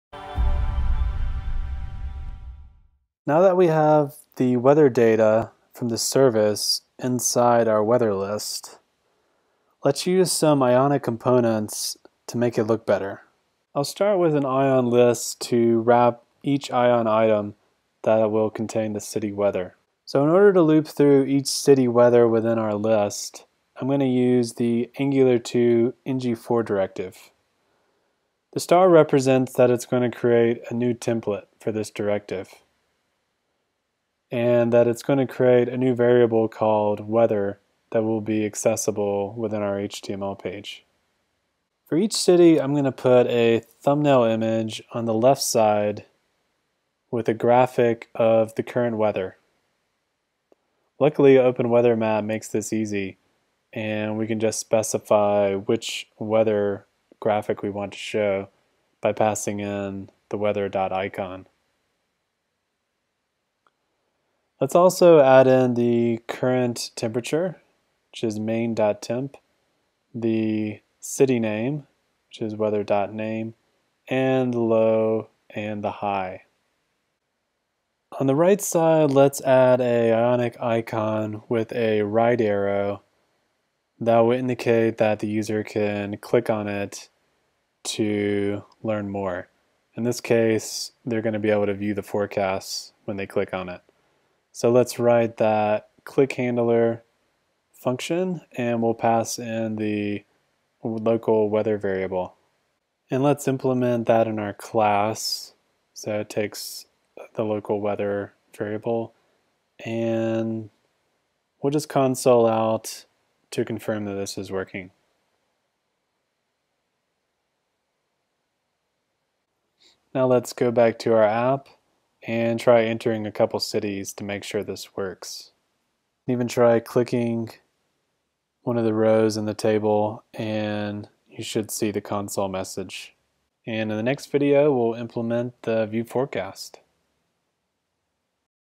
Ionic 2 - Weather App - 4
In this lesson we will:

Turn weather data into a visual representation
Show a list of weather using the Angular 2 directive *ngFor